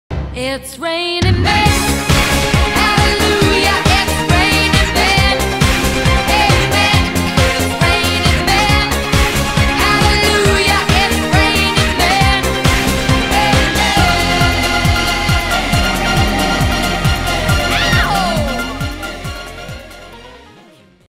Adobe After Effects Practice - Raining Men
Practice in Adobe After Effects with a new Green Screen.